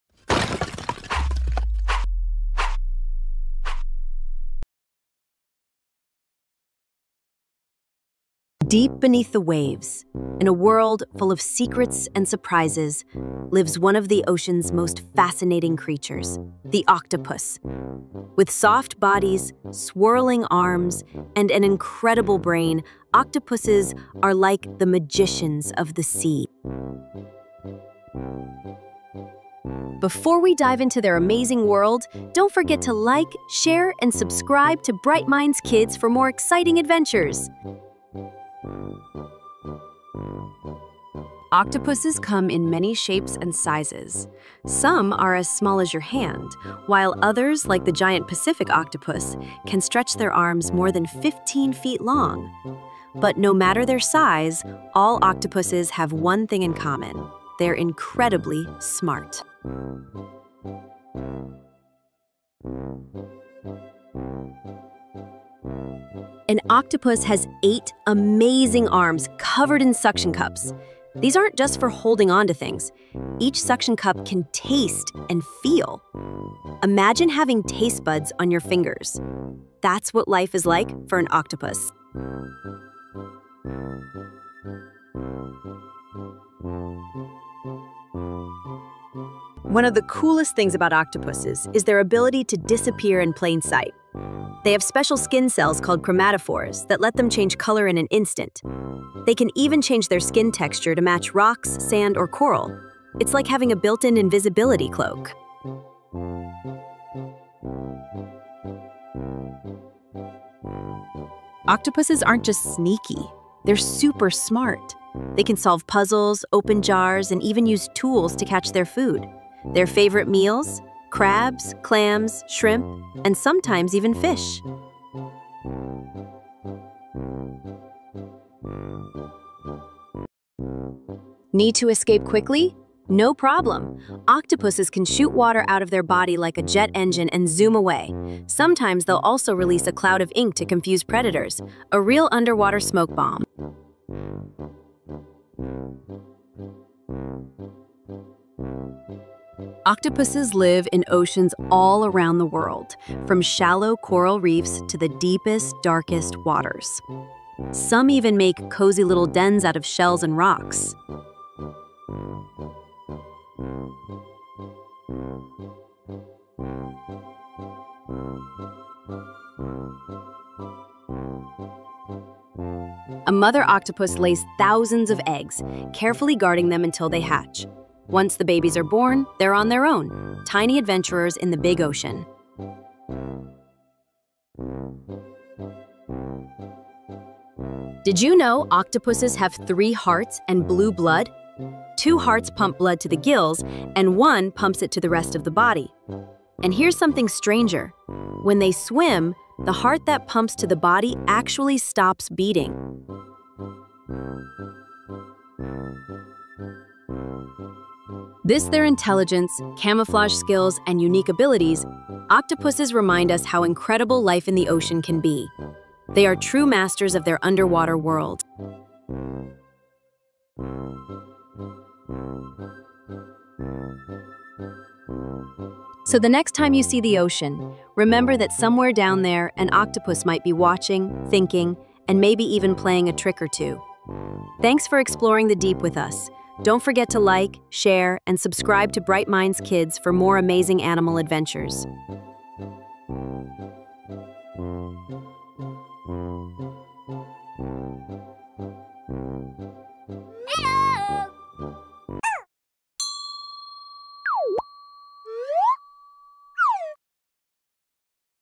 Meet the Amazing Octopus | Educational Video for kids
Dive deep beneath the waves and meet one of the ocean’s most fascinating creatures — the octopus!
In this kid-friendly documentary, BrightMinds Kids takes you on an underwater adventure to explore the mysterious and magical life of octopuses. From their eight incredible arms covered in taste-sensing suction cups to their amazing camouflage skills, jet-powered escapes, and super-smart hunting tricks, octopuses are the true magicians of the sea!

You’ll discover how they live, hunt, protect themselves, and even how baby octopuses begin their journey in the vast ocean. Along the way, you’ll learn fascinating facts about their three hearts, blue blood, and surprising intelligence.

Perfect for curious kids, ocean lovers, and anyone who enjoys discovering the wonders of nature. Let’s explore the deep together!